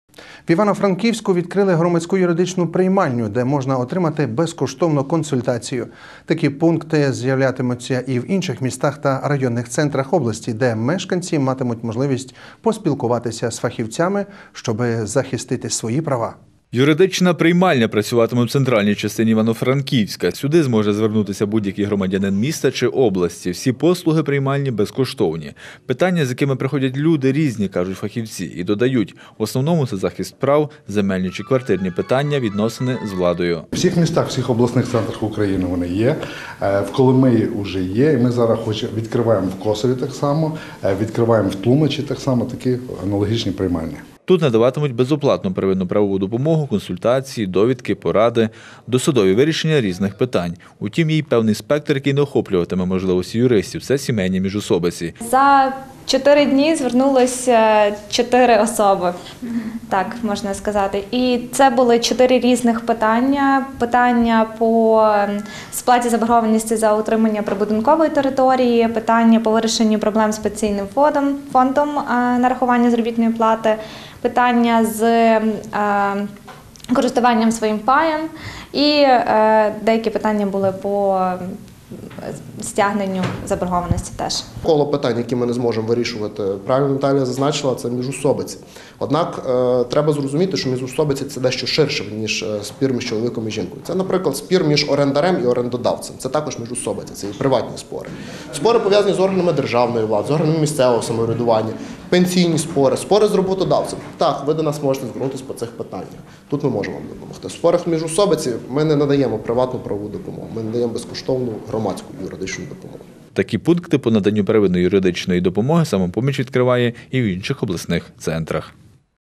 Розпочала роботу безкоштовна юридична приймальня
В Івано-Франківську відкрито громадську юридичну приймальню, де мешканці матимуть можливість поспілкуватися з фахівцями про захист своїх прав. Такі пункти з'являться і в інших містах та районних центрах області.